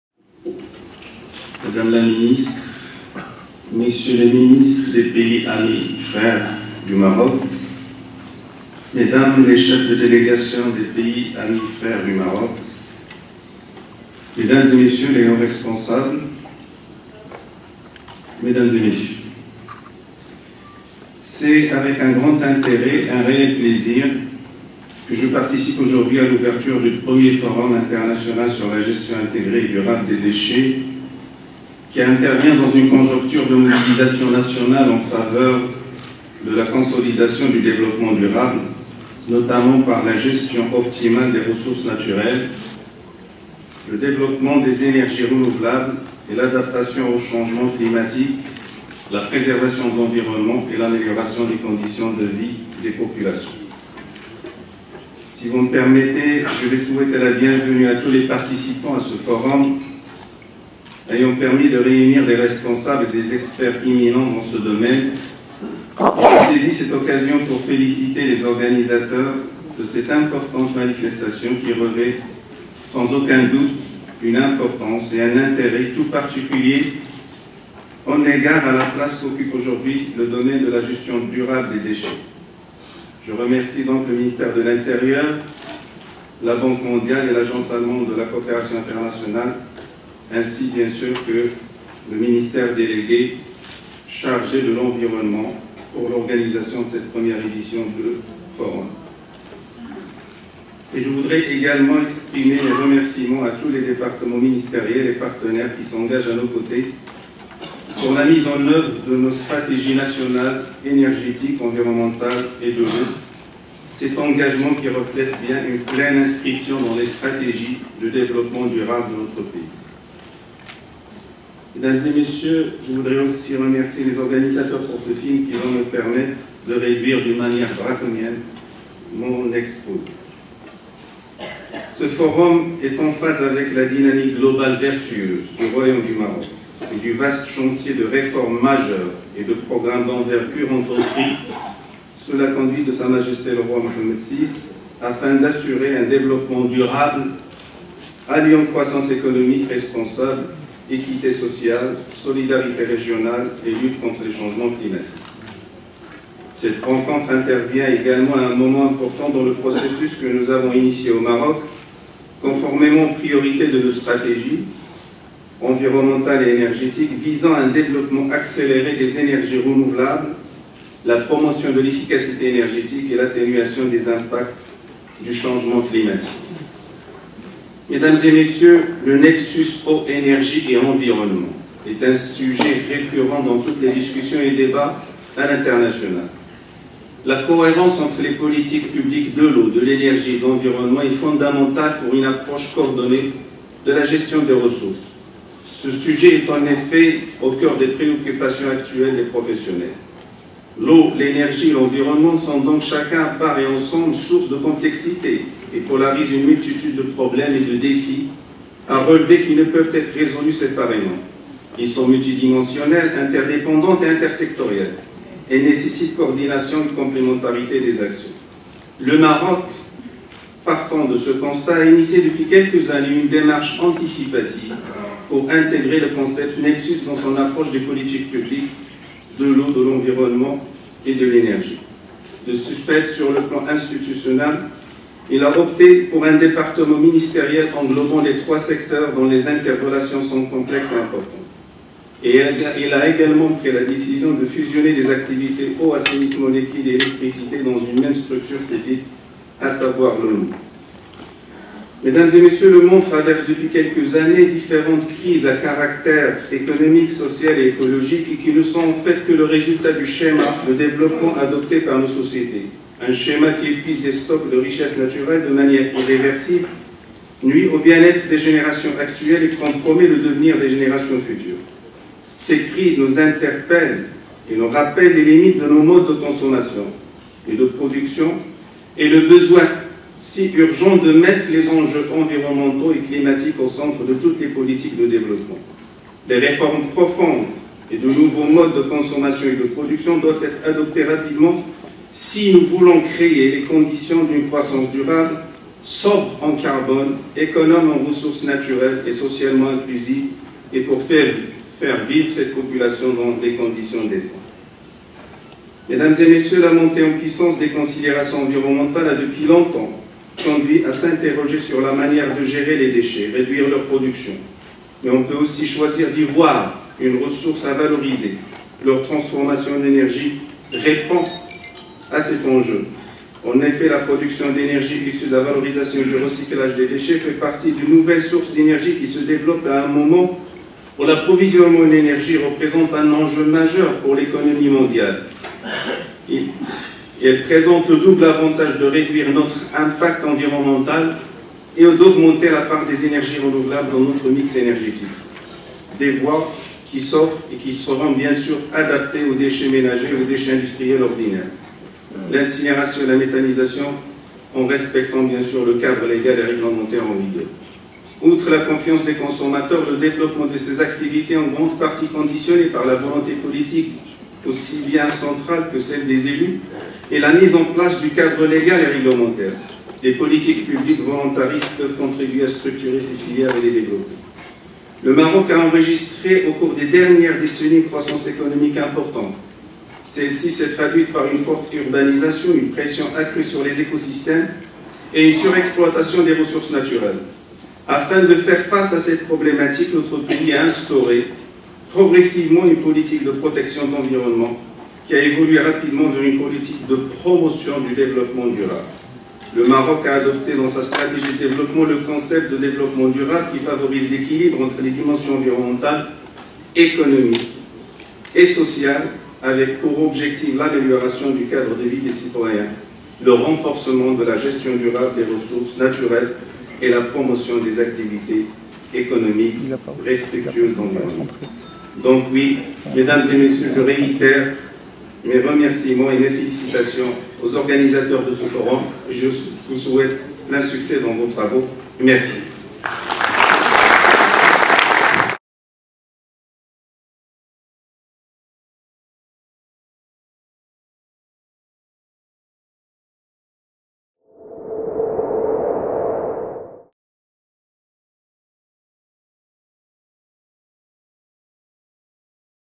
Abdelkader Amara: Allocution Forum Déchets Rabat Maroc - 20/06/2014
Allocution d'ouverture prononcée par M. Abdelkader Amara, Ministre de l'Energie, des Mines, de l'Eau et de l'Environnement, lors du Forum Déchets organisé à Rabat le 20 juin 2014.